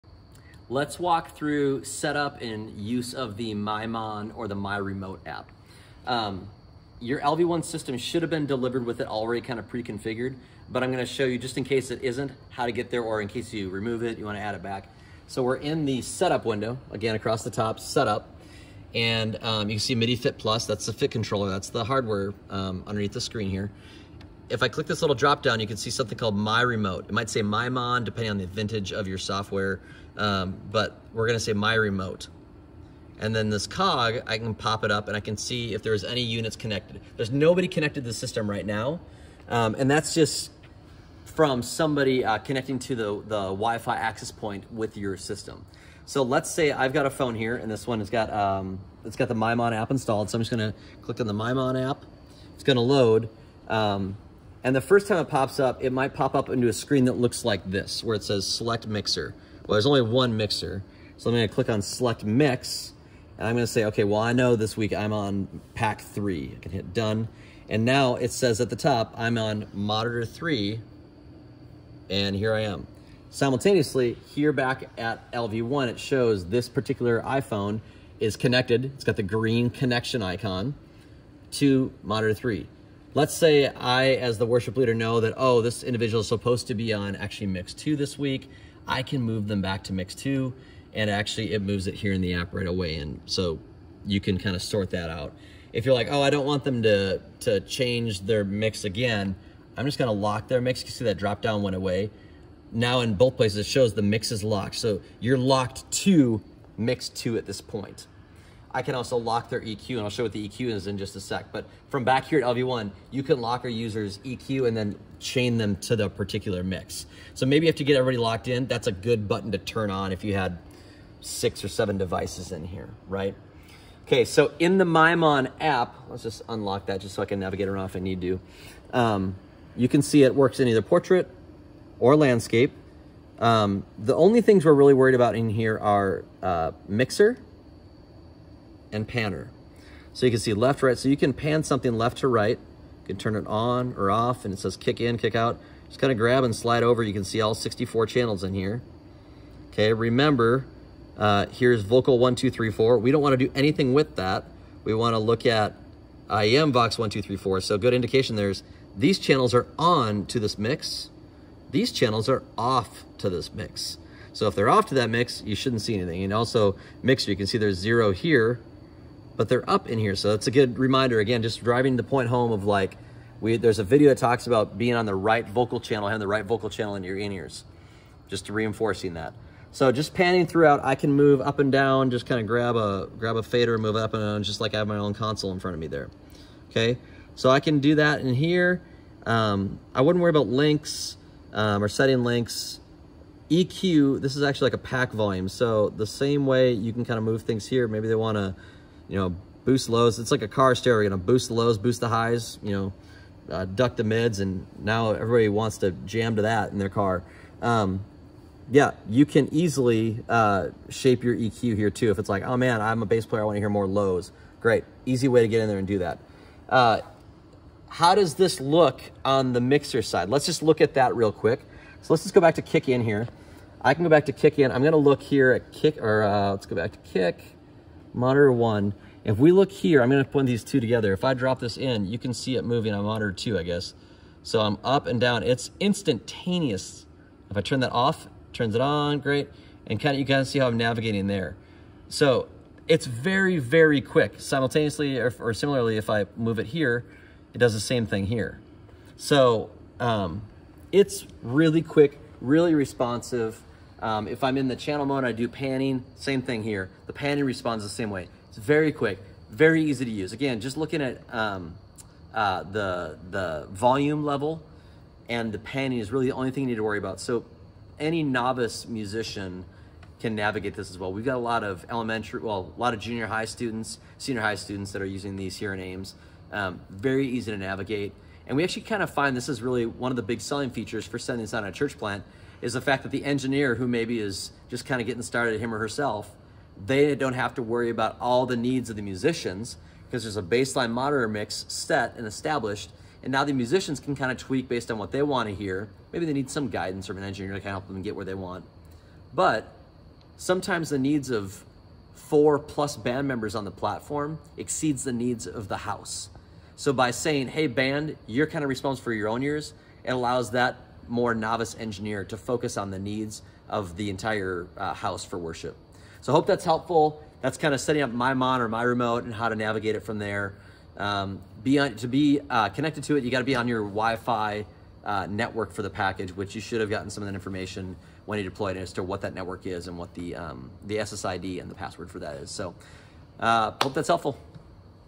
20 - Using the LV1 MyMon App
Using the MyMon IEM mixing app with your Salt Network LV1 system.